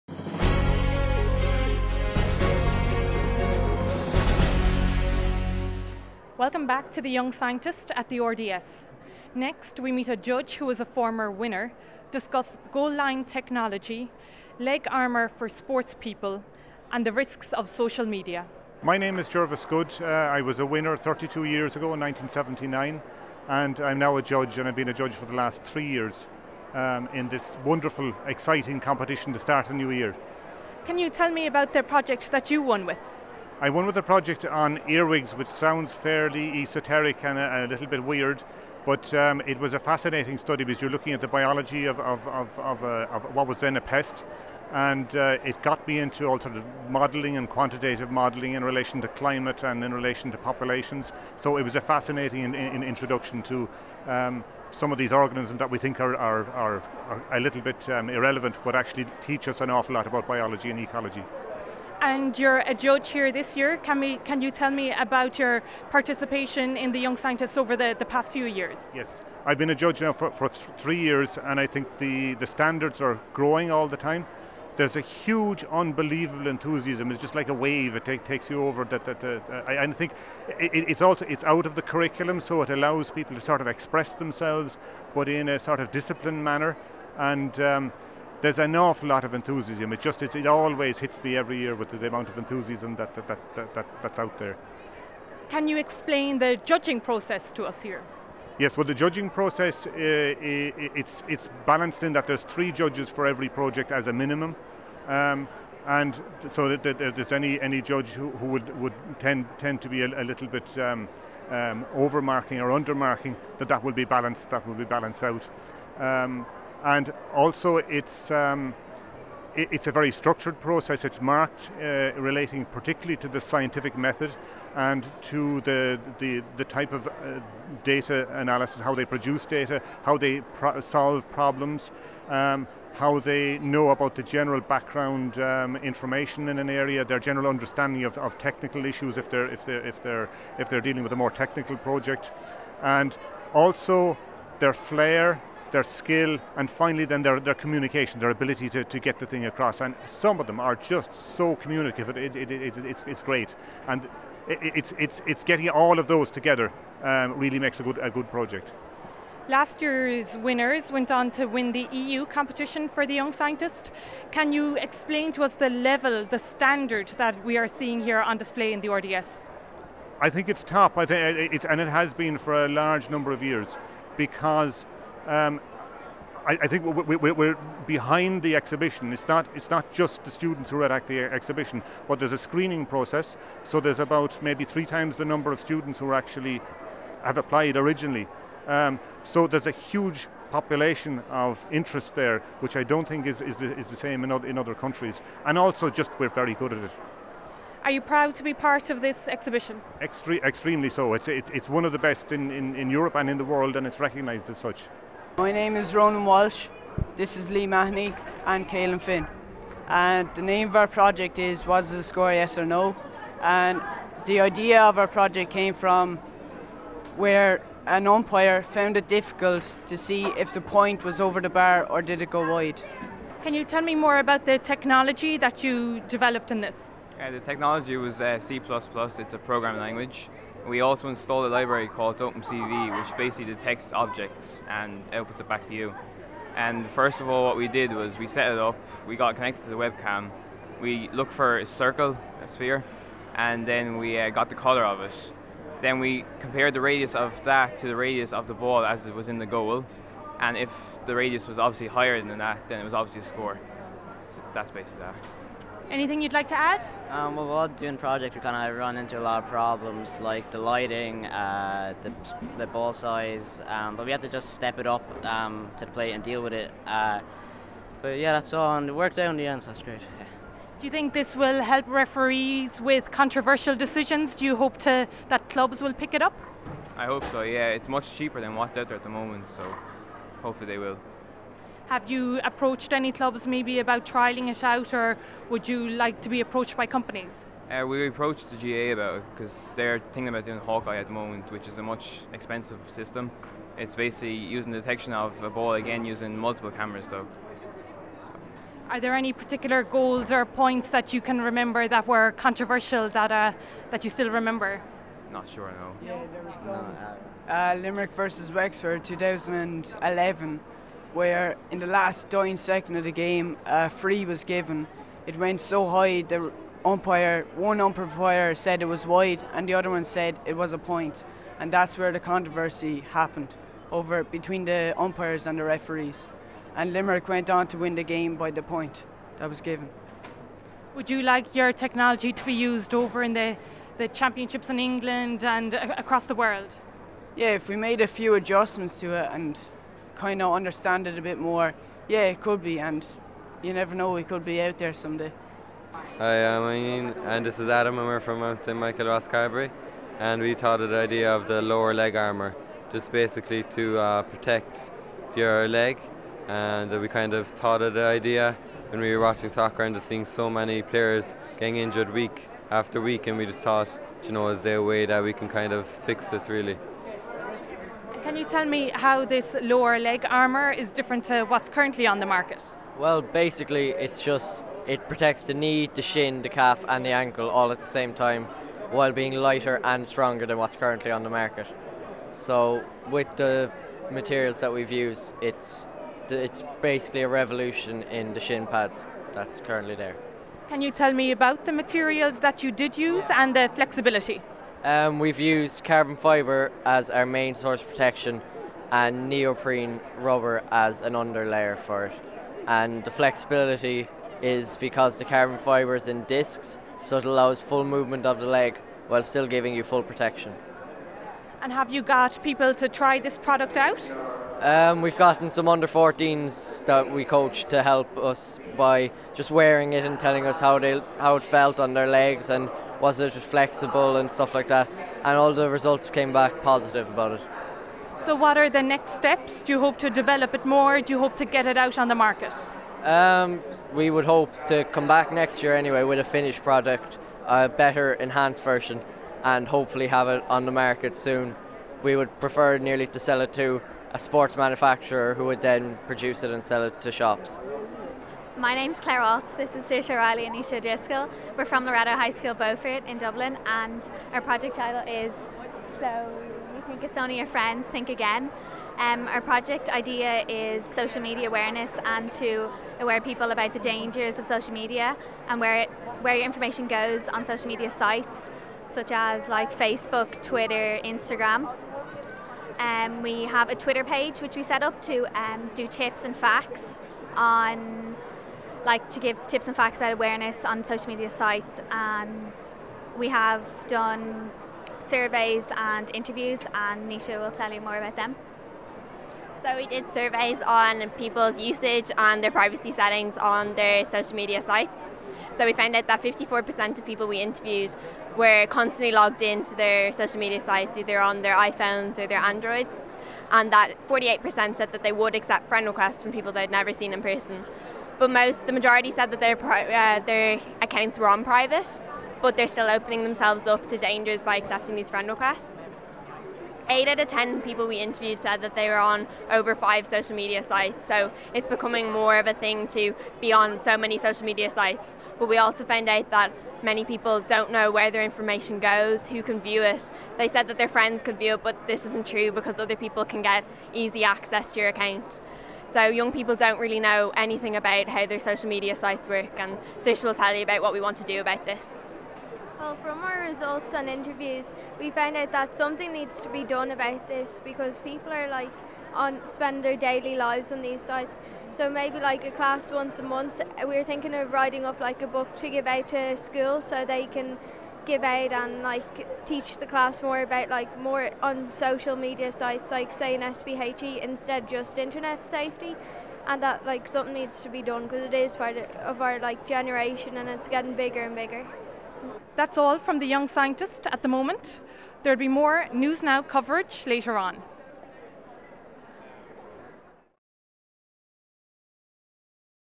BT Young Scientist and Technology Exhibition
Patricia O'Callaghan meets with some of the competitors at this year's BT Young Scientist and Technology Exhibition.